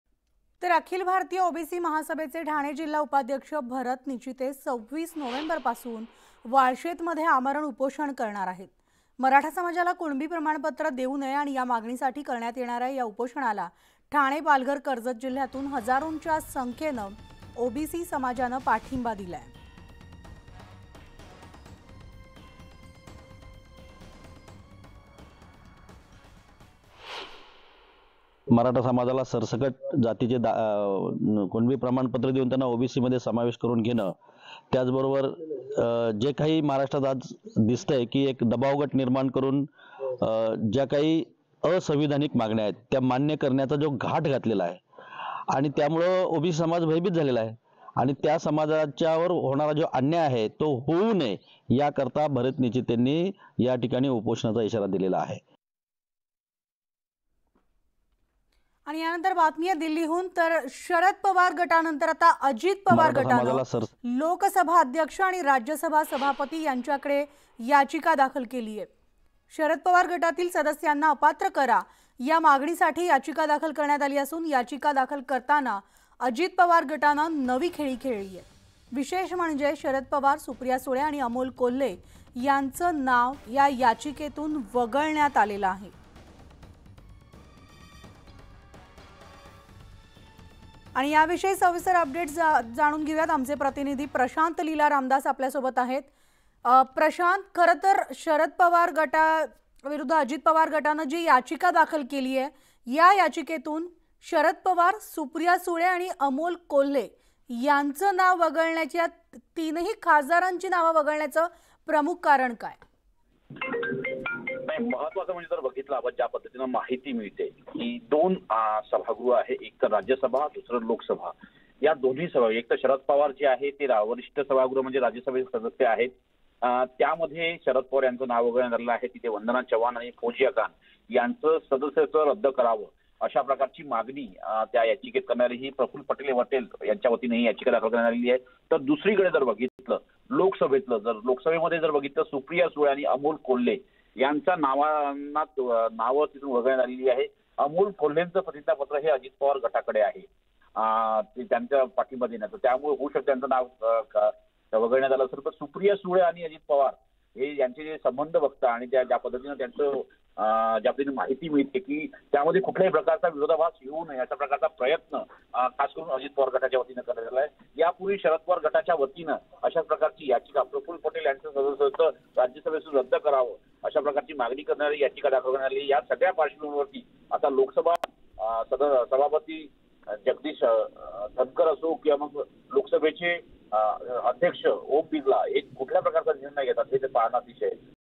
BREAKING NEWS | मराठा समाजाला कुणबी प्रमाणपत्र देऊ नये | Kunbi Certificate | Maratha Community
ओबीसी महासभा पदाधिकारी करणार उपोषण, २६ नोव्हेंबरपासून आमरण उपोषणाचा इशारा